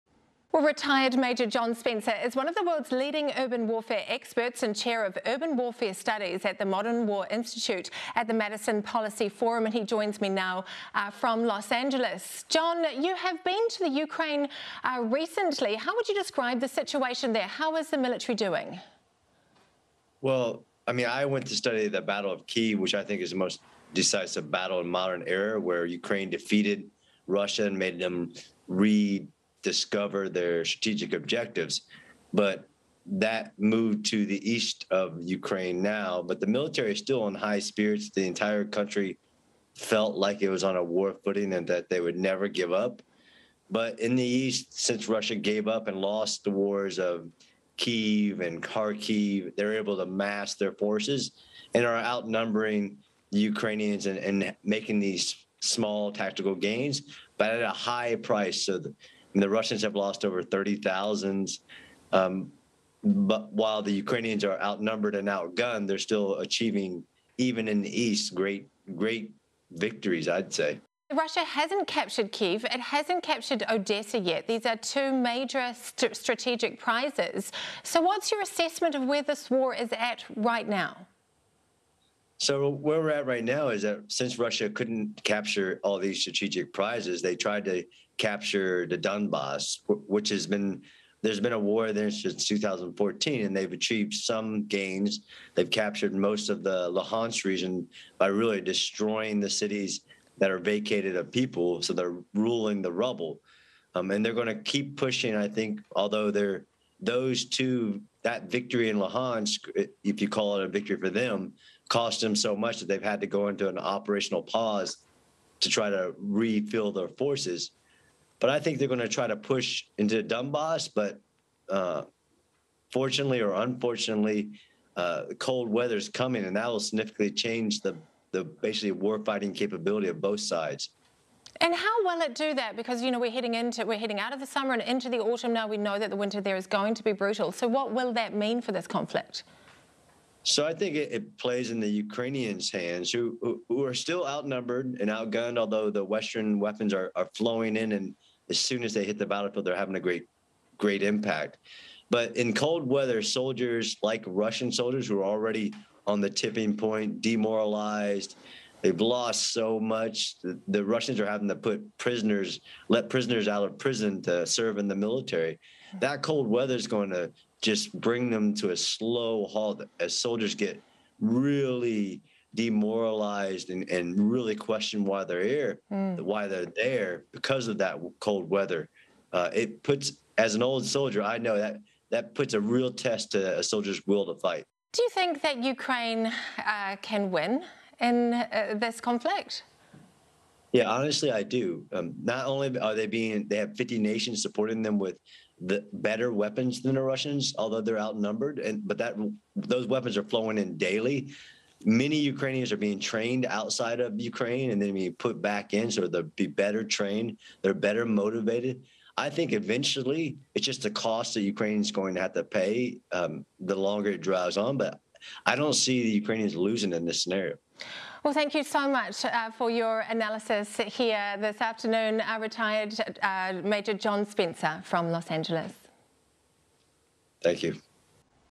Russia is 'ruling the rubble', but can Ukraine win the war? | Newshub
After Russia seeing some success in the Luhansk region, we ask what's next for the war in Ukraine?

Retired Major John Spencer, one of the worlds leading urban warfare experts and chair of urban warfare studies at the Modern War Institute, says "fortunately or unfortunately cold weather's coming", and it could bring already demoralised Russians "to a slow halt" - playing into the hands of "better trained, better motivated" Ukrainians.